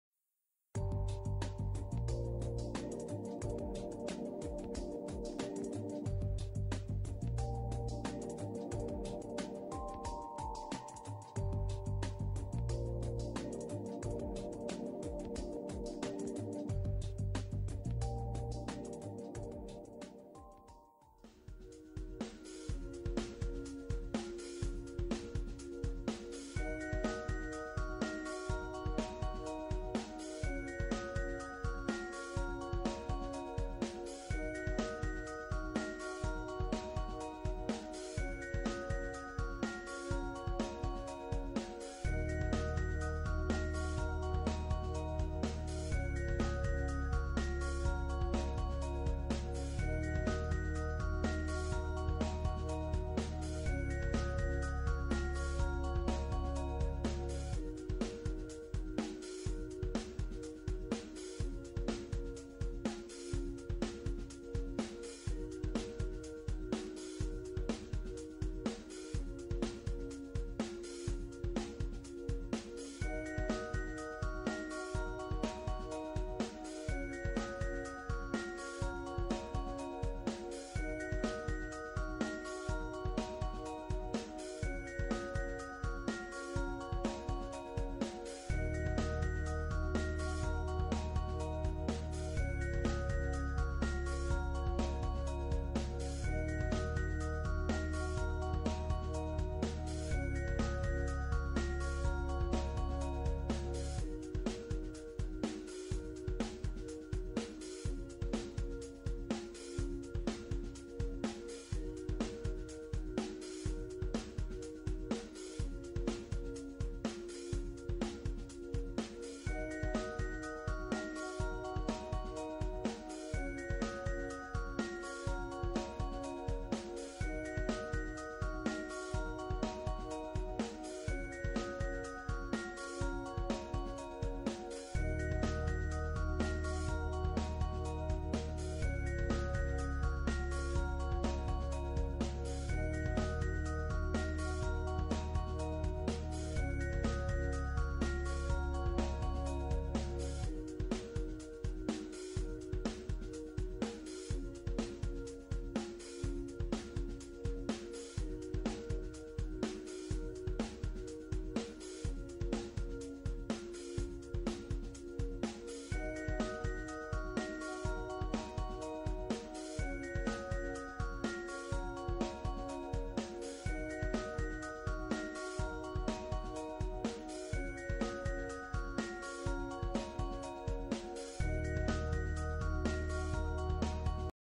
How to fix .DLL missing Error on Windows
In this video i told how to fix .dll error in windows